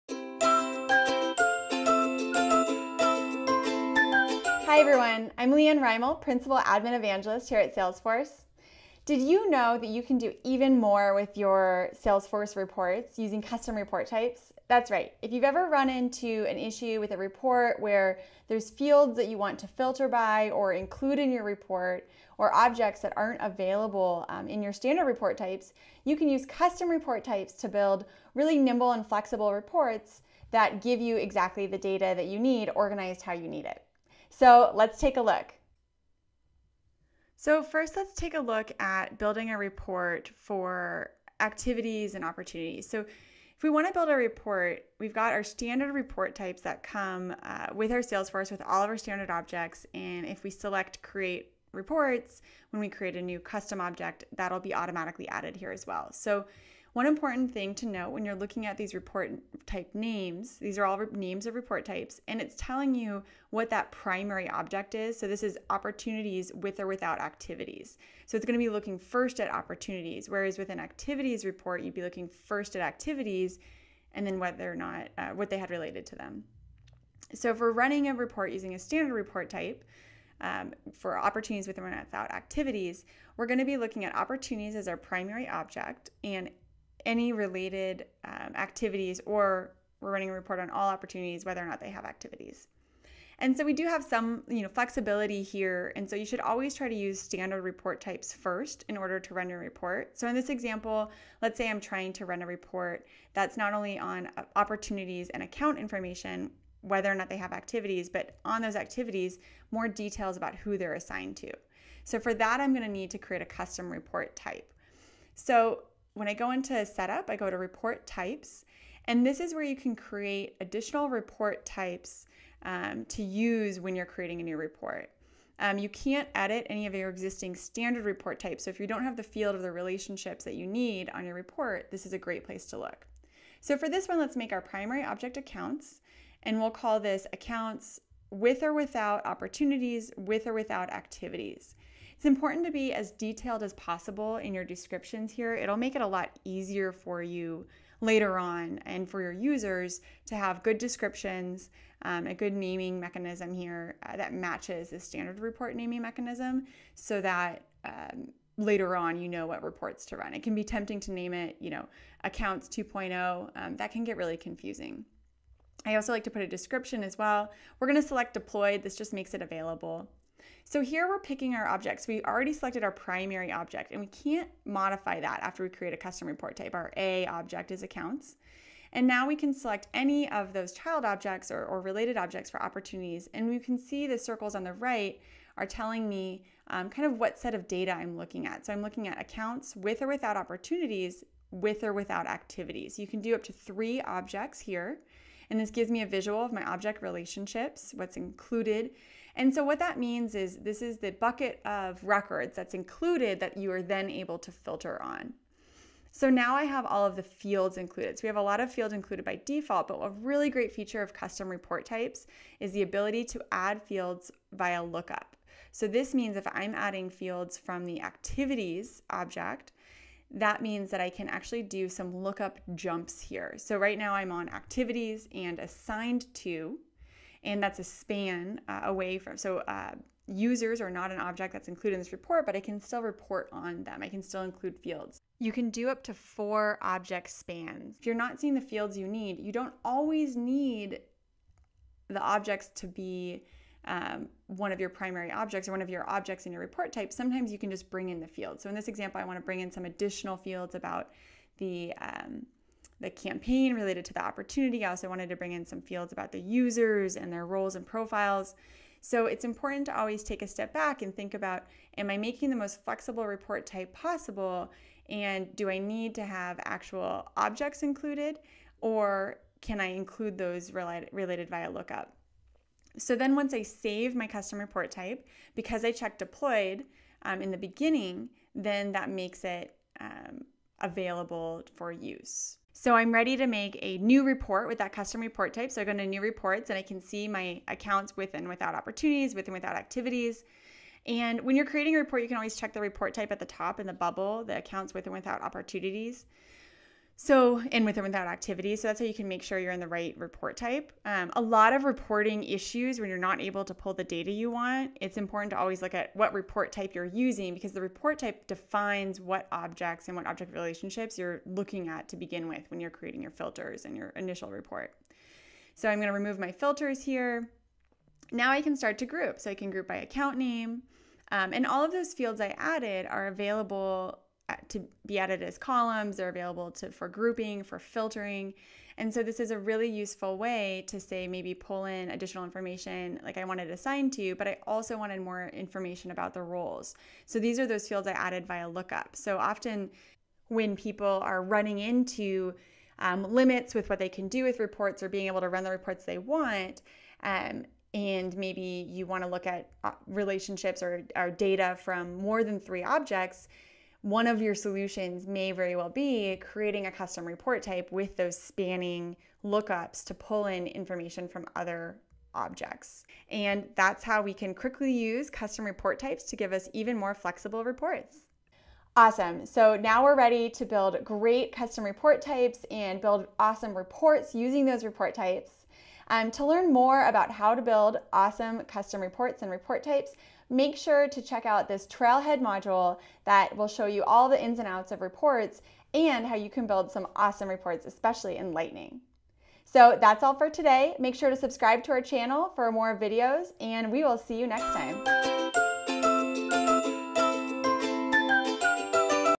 Use Custom Report Types to Build Powerful Salesforce Reports
Reports & Dashboards are a powerful way to understand and visualize your data, and our reporting starts with Report Types. When you create a report in Salesforce, you first select the type of report you will create. Often the standard report types will meet your reporting needs, however sometimes we run into reporting limitations with our standard set of report types. If you aren't able to create the report you need with the standard report types available, we can explore Custom Report Types.